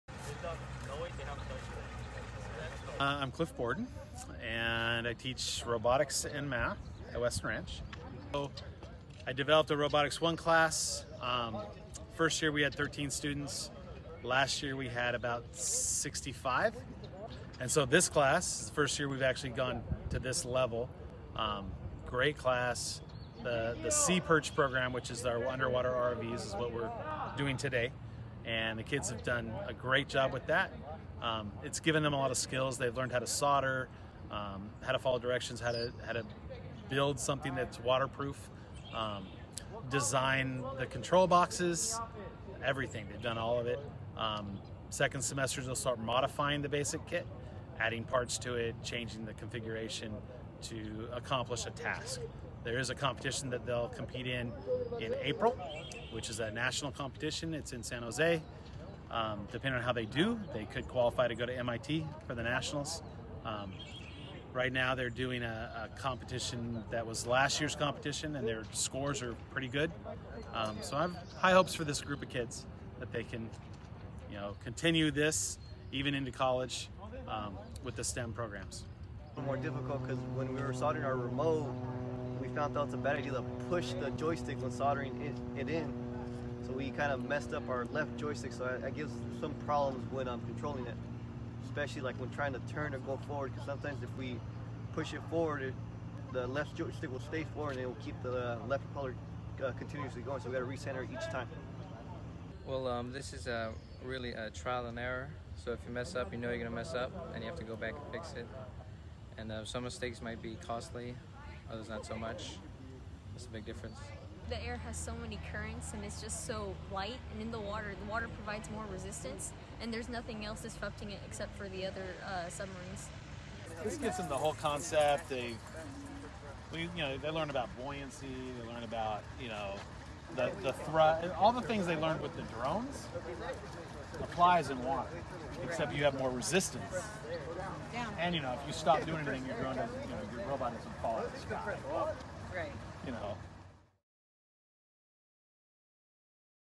Weston Ranch Robotics
Cliff Borden and his WRHS robotics students move forward with their SeaPearch efforts. At this stage, they are learning basic maneuvers and will soon be adding more complexity to complete a variety of underwater tasks.